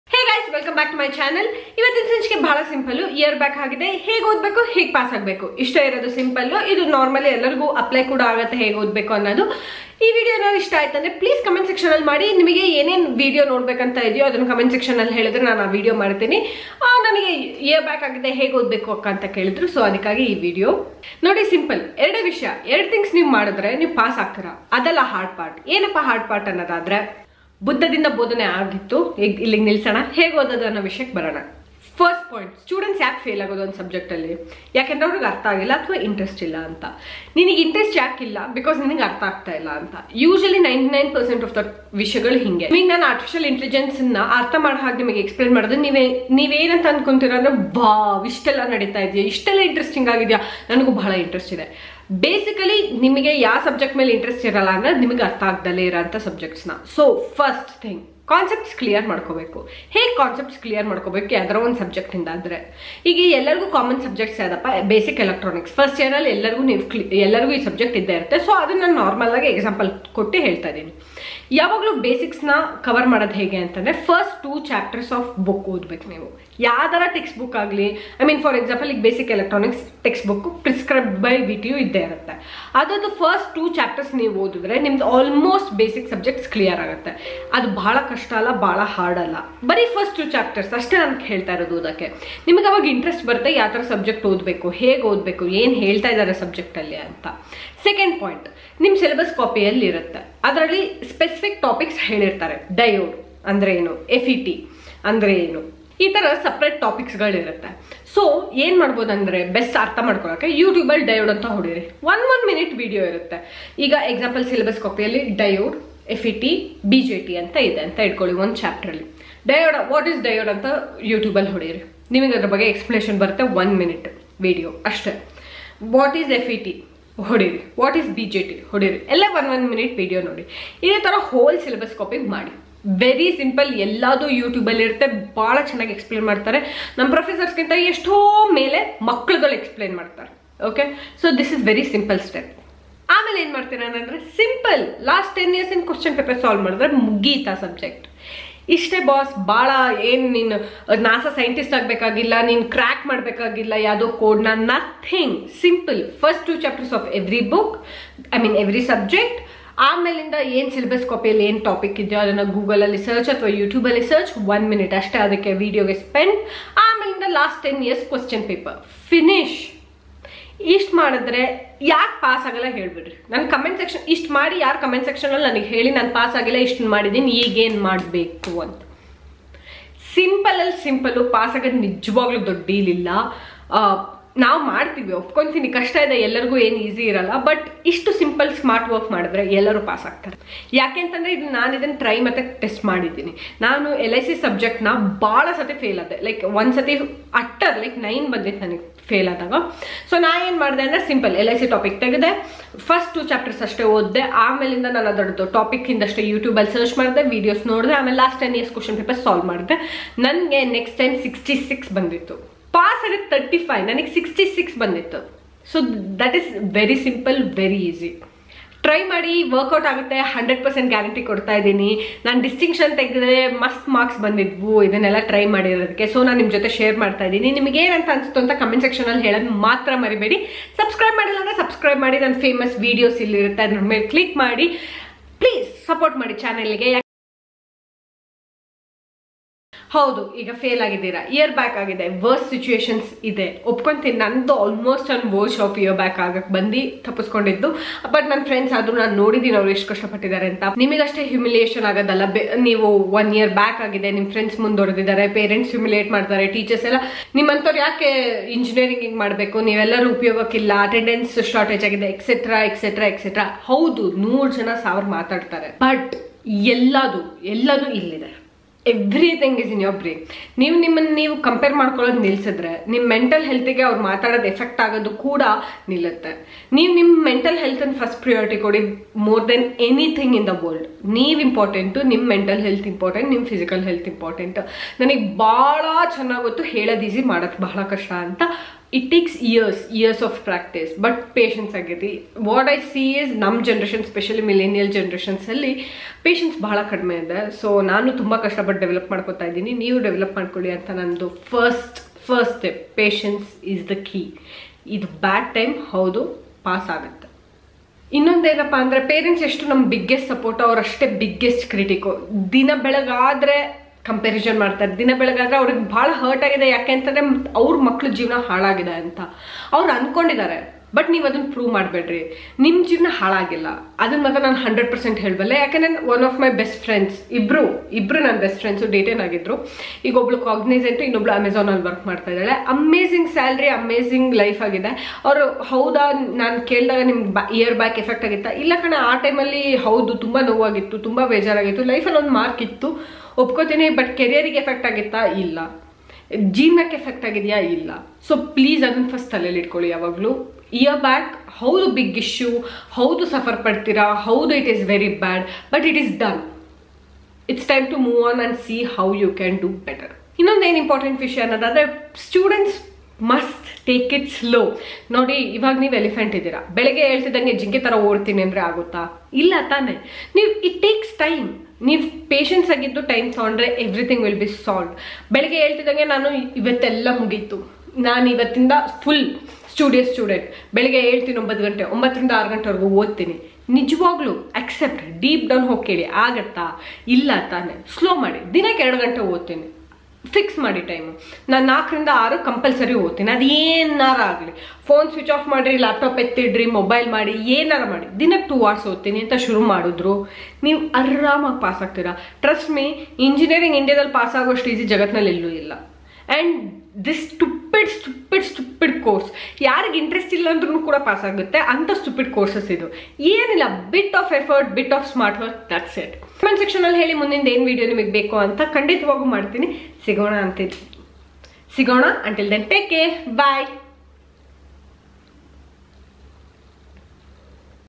YEAR BACK IN ENGINEERING ? How to study? 2 SIMPLE STRATEGY
2  simple strategies to pass exam for year back students. I hope it is helpful to you all. Please let me know how you feel in the comment section below.